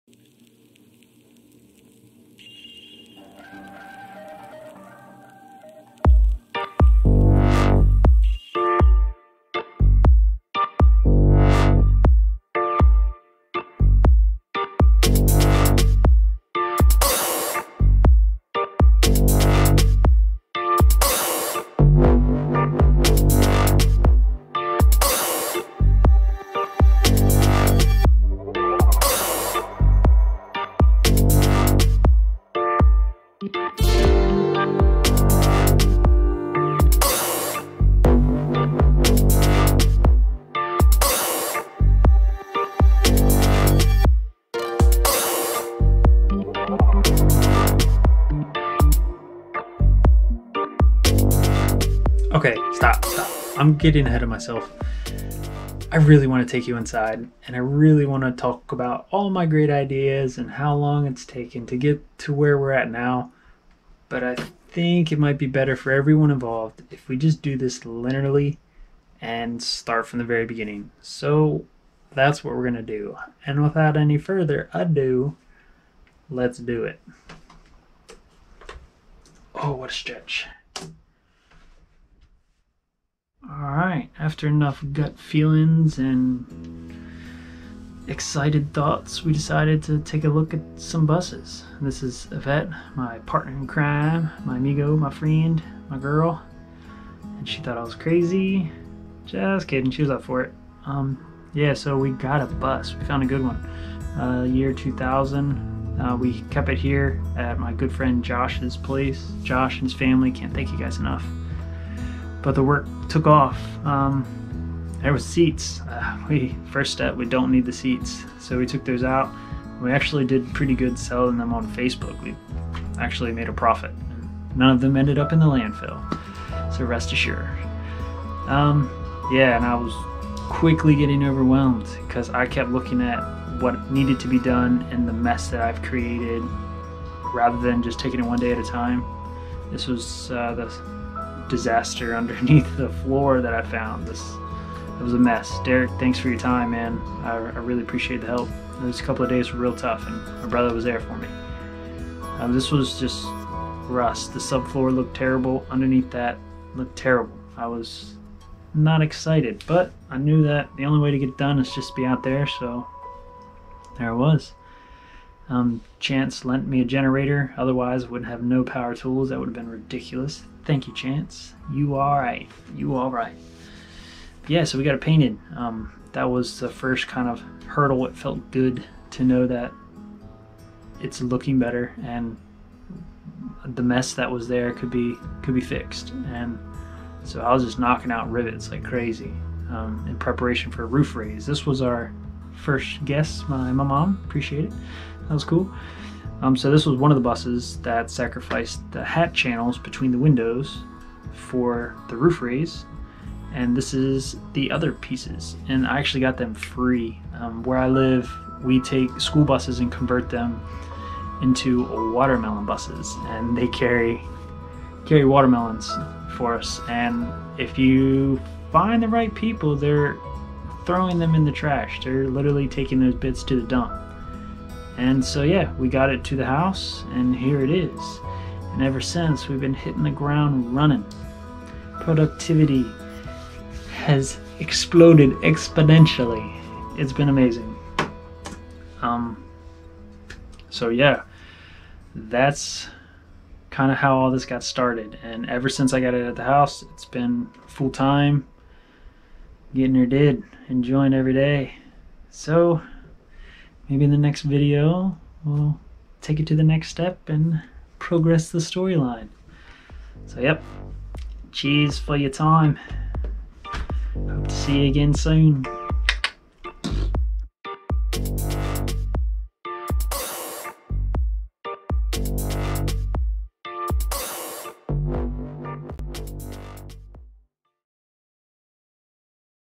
No More Hiding the Bus
So I know I have not shared any progress on the bus since it was purchased but It has been full on since day one. I have documented the process along the way but by the time I'm done working on the bus for the day I'm ready to change topics. But with a recent upgrade of recording equipment (thanks heaps Derek) I feel more interested in filming. This will be the first of many.
If only a teaser, at least it is a taste ;)

Editing program: Filmora
Recording device: Gopro 8 Black
Photo devices: various cheaper phones haha